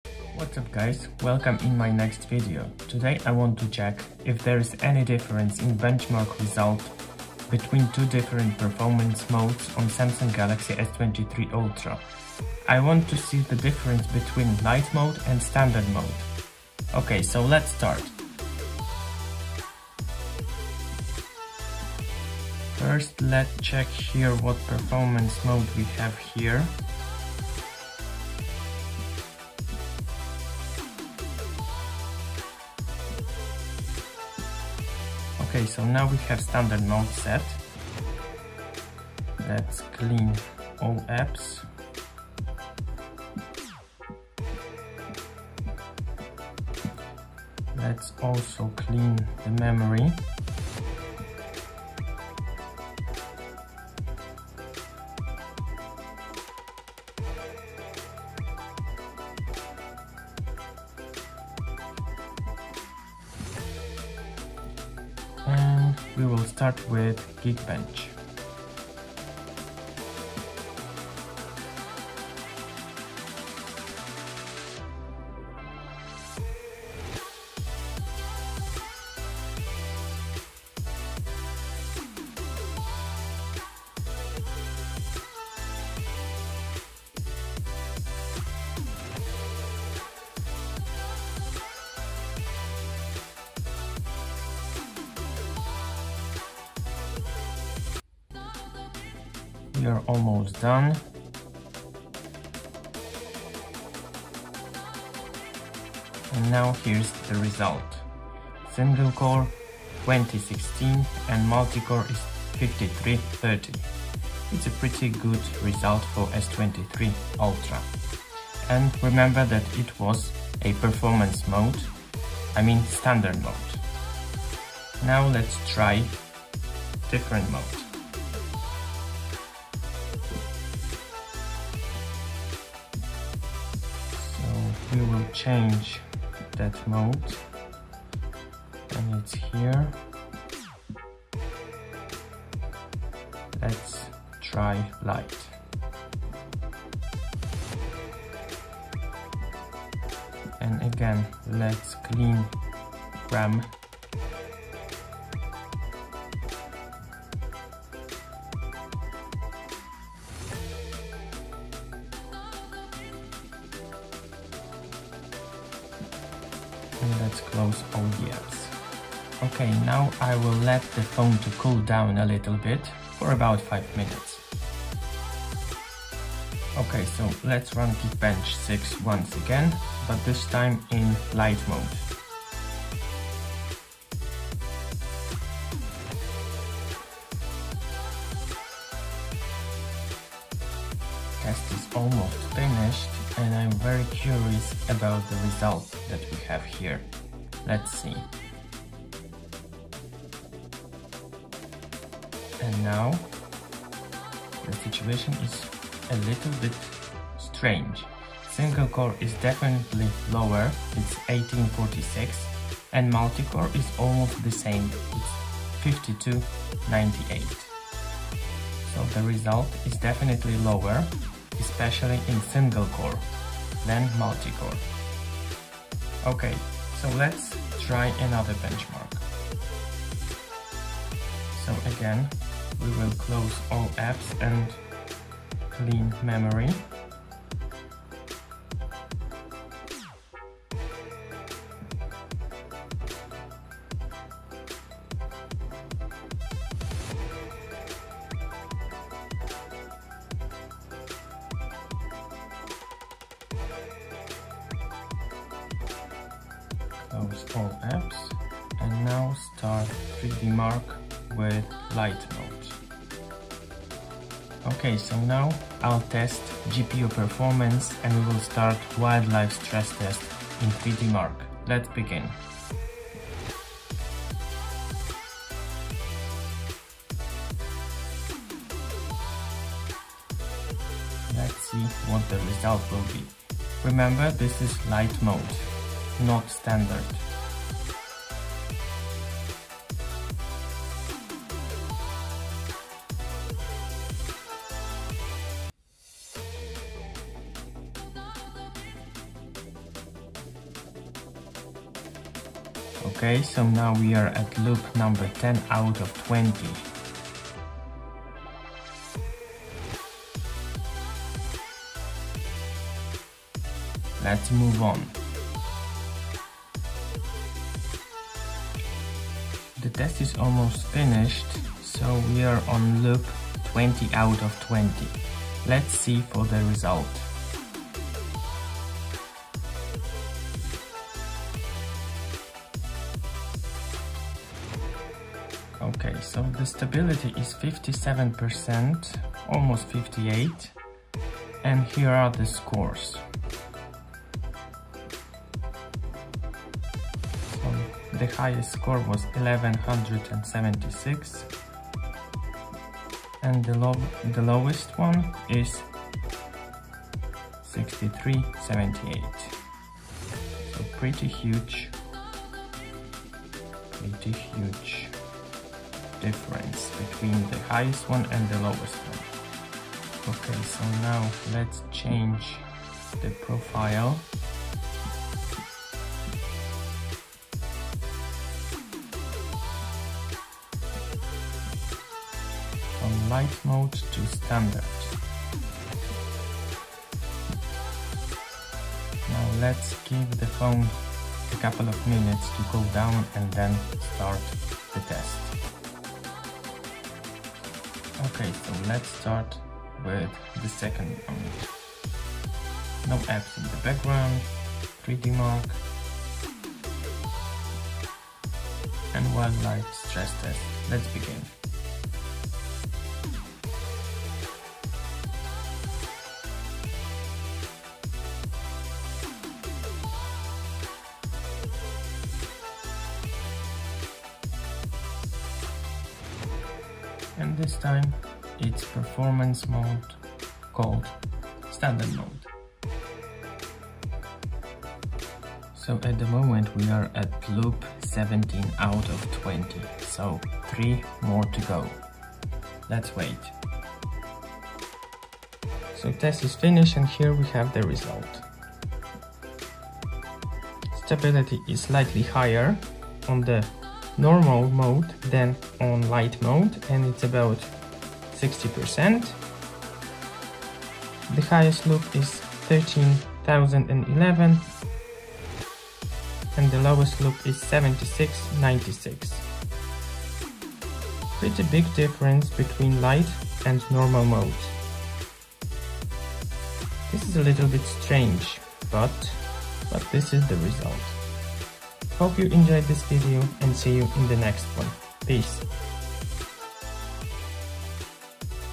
LIGHT MODE OR STANDARD MODE? | Benchmarks results on Samsung Galaxy S23 Ultra. 🤔
In this video I'm going to compare two modes in my Samsung Galaxy S23 Ultra. Light performance mode and Standard mode. Let's see which mode will get better results in two popular benchmarks. Will there be a huge difference in favour of Standard performance mode or maybe other way round.